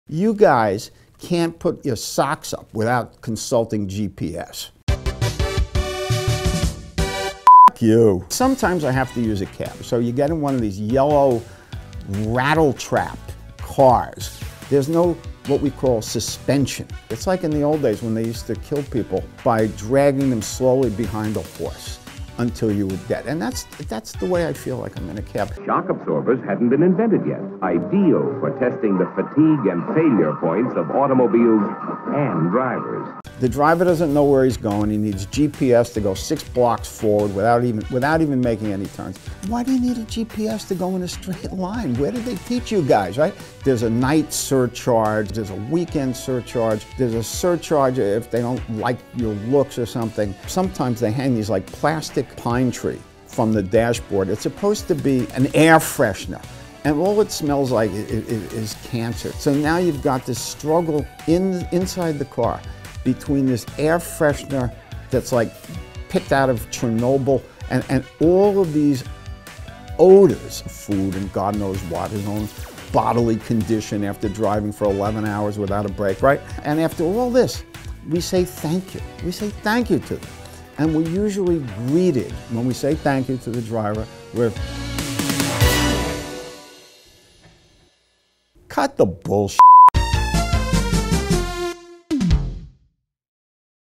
Febreze can't mask the stench of a bad taxicab driver | New York Post
In this edition of "Cut the BS, with Cuozzo," the New York Post's very own Steve Cuozzo pops off about NYC's classic yellow taxicabs. Between air fresheners and surcharges, his biggest beef is the lousy driving.